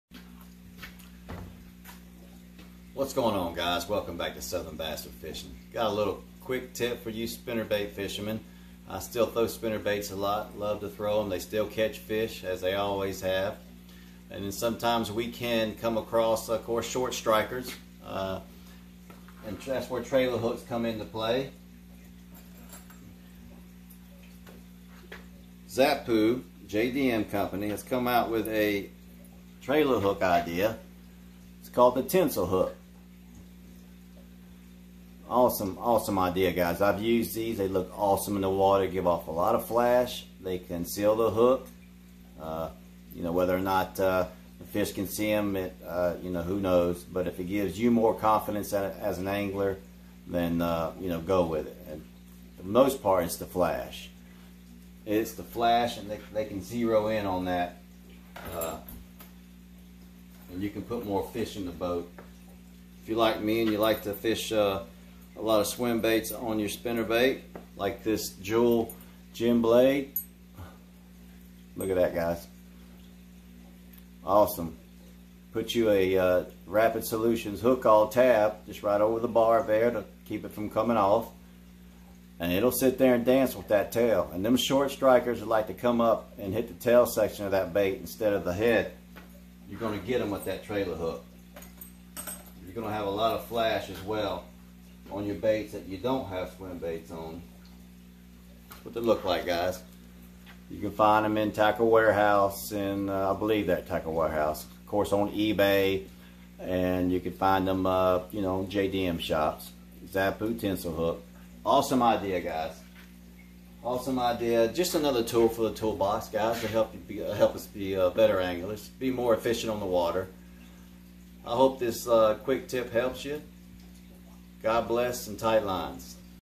Awesome trailer hook hack for Bass fishing!
A new spin on trailer hooks for Spinnerbaits and buzzbaits!